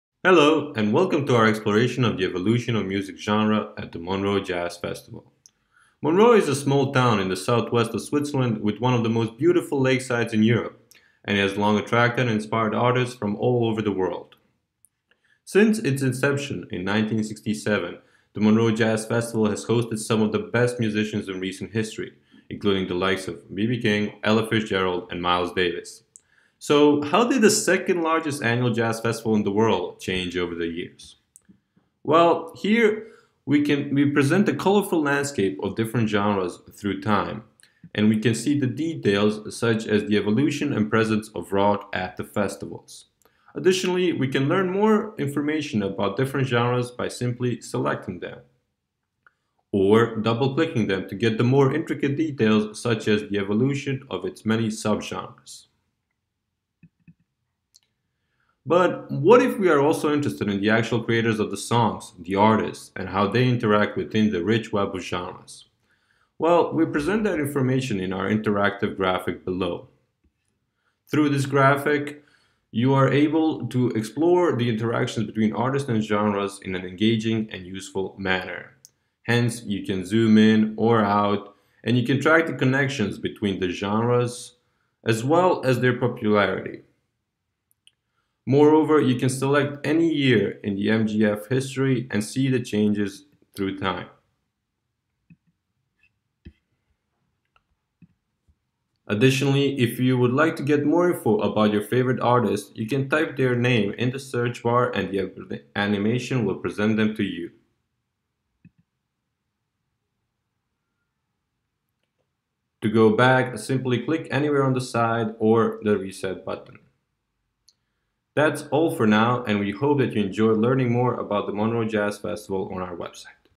Montreux Jazz Festival - Landscape of Tunes
We present our insights on music genre evolution at Montreux Jazz festival. In this screencast we demonstrate the elements of our visualizations, as well as provide more context in the story of MJF.

This is part of the project of Data Visualization (COM-480) at EPFL, Fall 2017.

Authors: Dino Mujkic, Sebastijan Stevanovic and Viktor Sanca